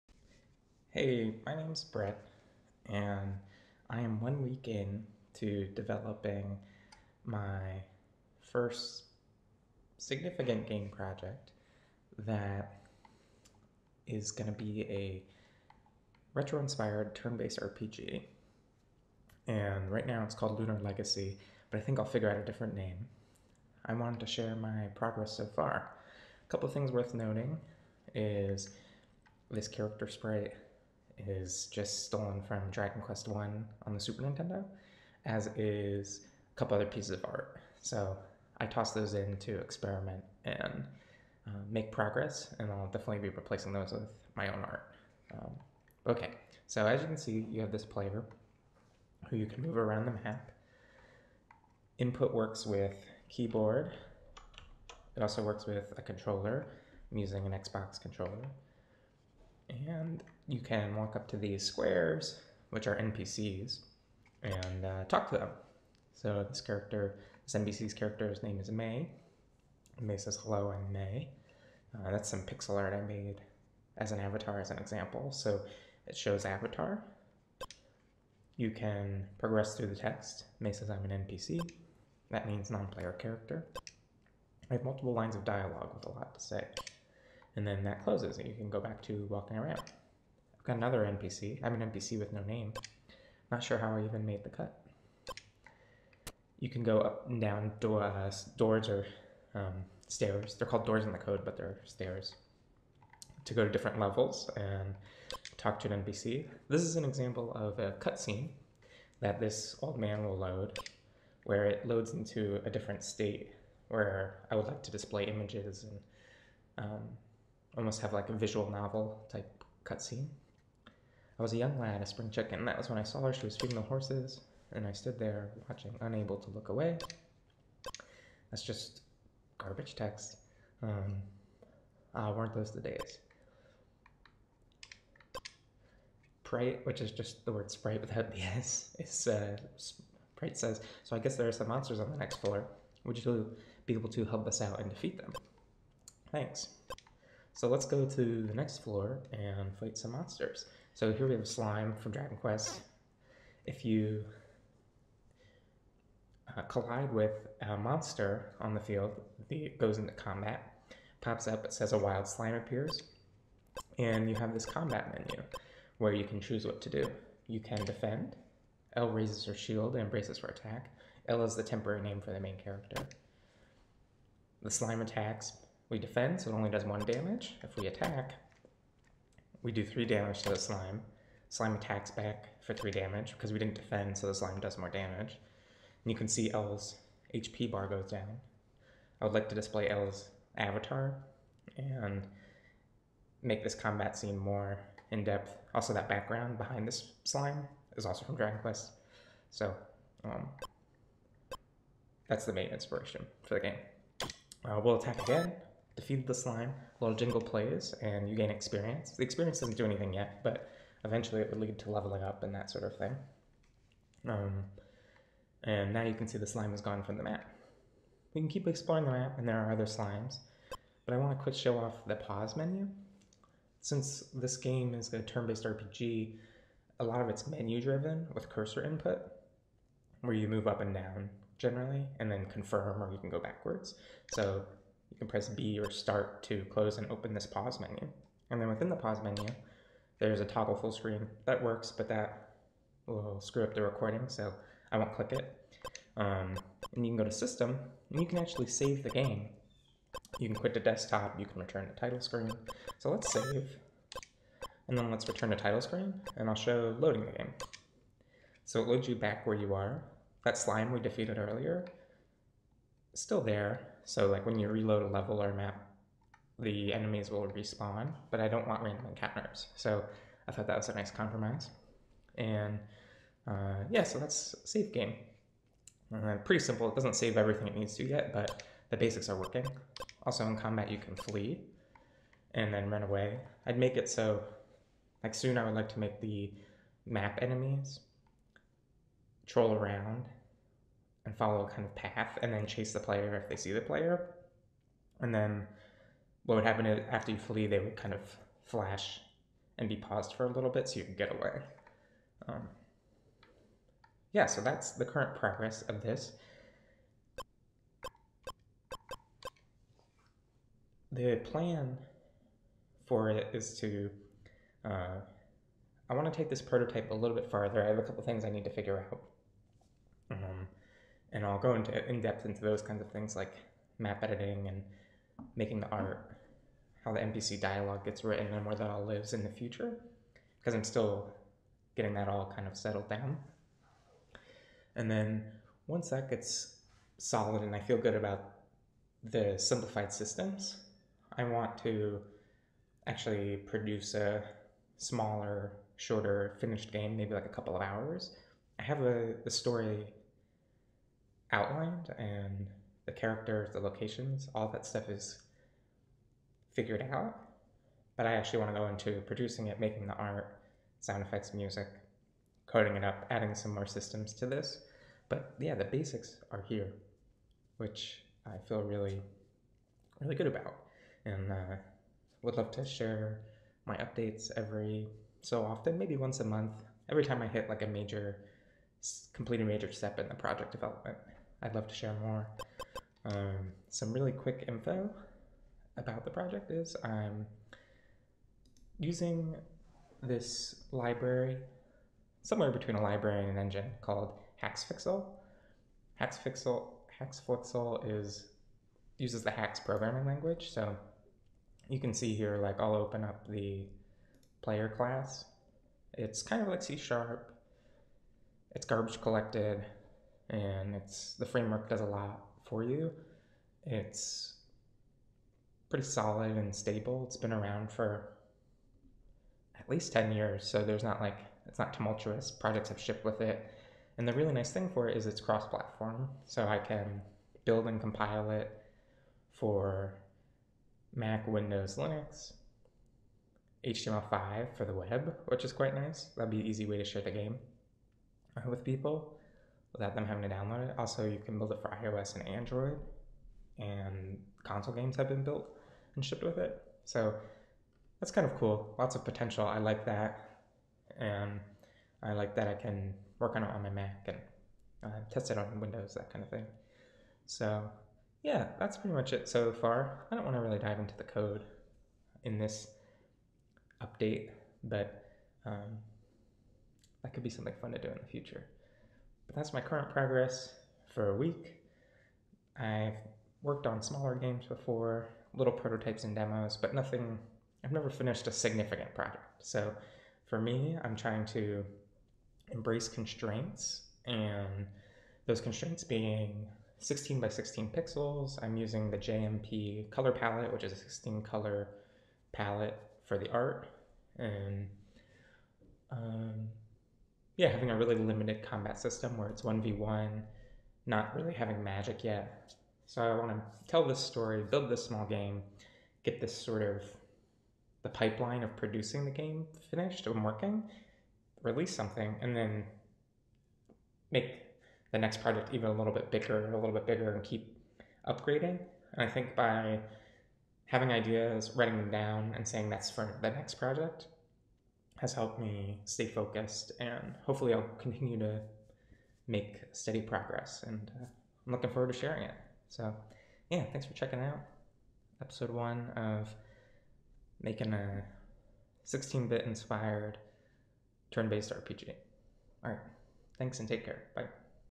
Elegian Devlog Episode 1 - A Week of Progress on my RPG
I spent the last week prototyping a turn-based, retro RPG inspired by games like Dragon Quest. I had a lot of fun with it, so I thought I'd share my progress!

In this video, I go over what I've gotten done so far so far—loading maps, NPCs with dialogue, combat, and menus.

The prototype doesn't really have a name yet, and it's mostly just an experiment while I prepare to make a small RPG using these foundations. My intention is to build a ~three hour game that tells a pretty simple story with two boss fights. I want to keep the scope small and finish and release it.

I can't wait to share my progress as I continue to work on the project. I'd like to cover the entire process, from writing the story, to making the art, to code, to managing the project.

My dream is to make and release games of increasing complexity as I get better and better as a game developer.

It's worth noting that the monster sprite, player sprite, and combat background are all from Dragon Quest on the SNES. I'll be replacing those with my own art once I start working on the actual game!

Thanks for checking out my progress and following along! I'd love to hear about your progress on any games you're playing or making. Cheers!